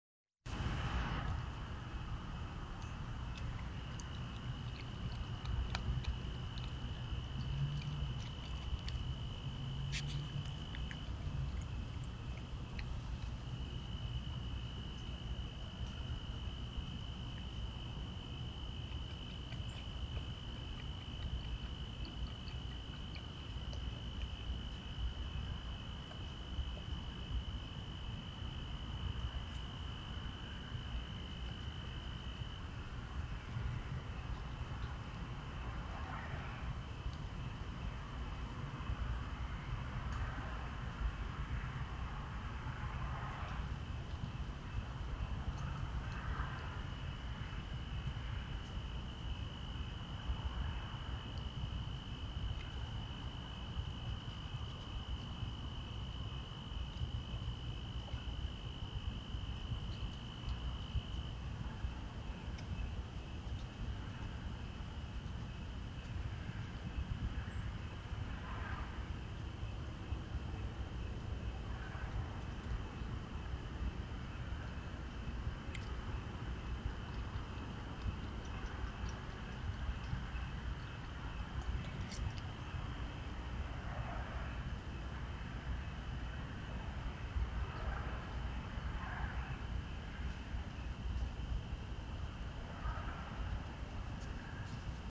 Opossum Disappointment Oct 14 2024
Critter Cam Daily has noted that the base state of Opossum is disappointment, despite their being Awesome. When you arrive in the wee hours of the morning after all the yummy Noms are snacked by Foxes and Raccoons and other Opossums, disappointment is natural. Good think Opossum is used to it. Come again, come earlier. Bananas again tonight for the early arrivals.